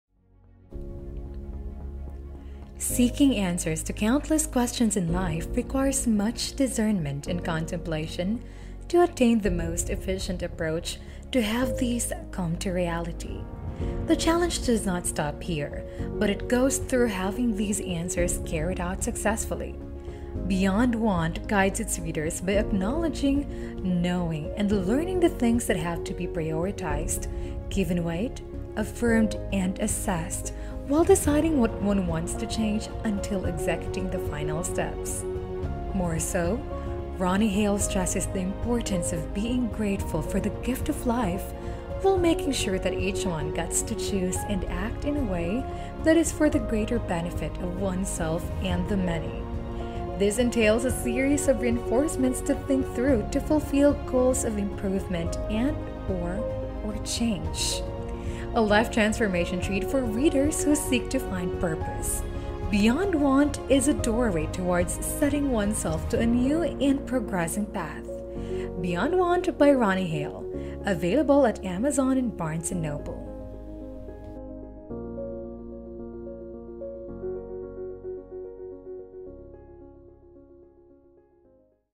Beyond Want Ronnie Hale Book Video Trailer
If you aren’t where you want to be in life, it may have something to do with you. What you want, think, how you spend your time, and how you feel are all influenced by external variables that can invade your inner world. When this happens, the results often lead to disaster. But by rethinking currently held beliefs and deciding what is truly important, you can work toward your best life. Now is the time to literally retrain your subconscious mind. In this book, the author explores how to prioritize your time and make positive decisions each moment of every day. Learn how to: • see all the options available to you on a daily basis; • ask yourself new, better, and empowering questions; • redefine, reshape, and reimagine your life; and • map out the steps you must take to achieve your goals. Hale also examines how prayer and affirmation is a conduit between humanity and the source of creation and how action steps are like stepping stones leading us to where we want to go. Look at life with a fresh perspective and start living your best life now—without anything extra—with the lessons in Beyond Want.